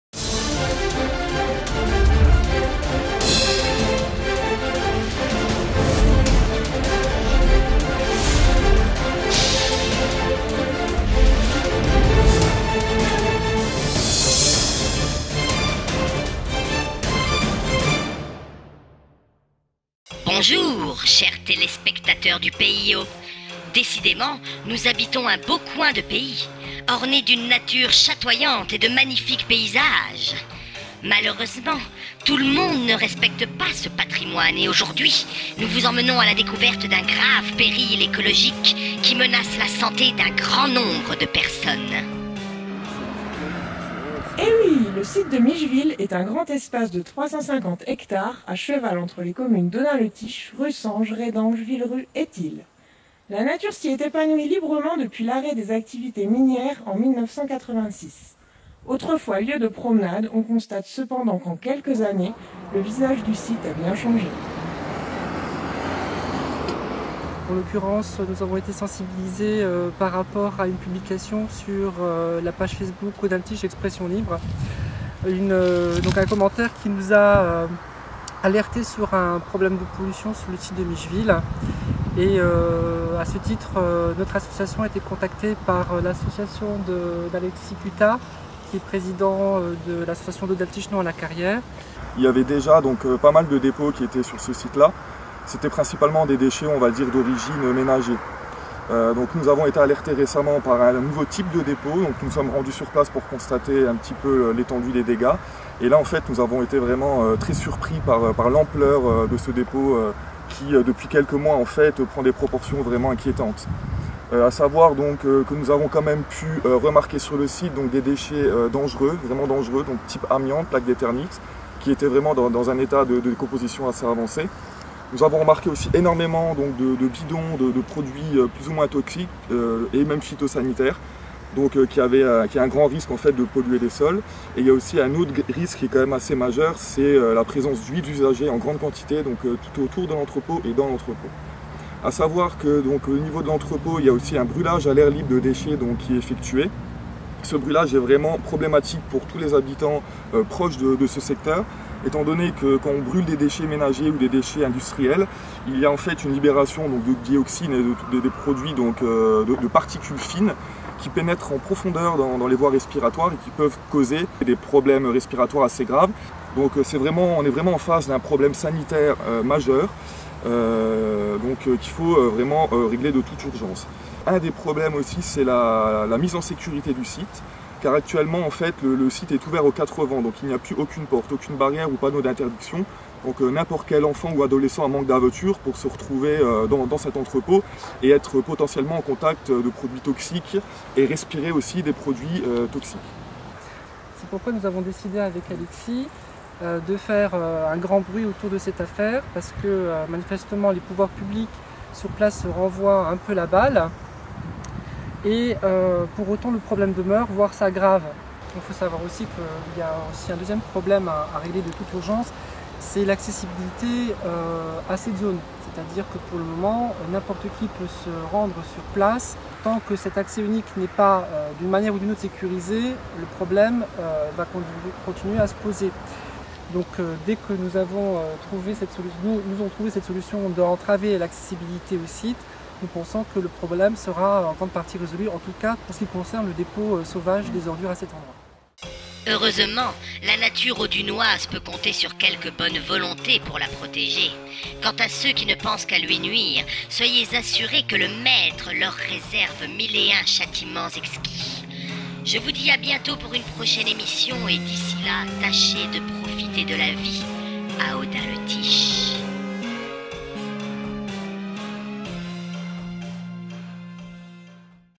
Scandale Tiche #1 | Micheville, éco-décharge?
Scandale Tiche s'intéresse aux sujets qui touchent directement la vie des habitants d'Audun-Le-Tiche et environs. Aujourd'hui nos caméras se braquent sur les importants dépôts d'ordures sauvages qui empoisonnent le site de Micheville...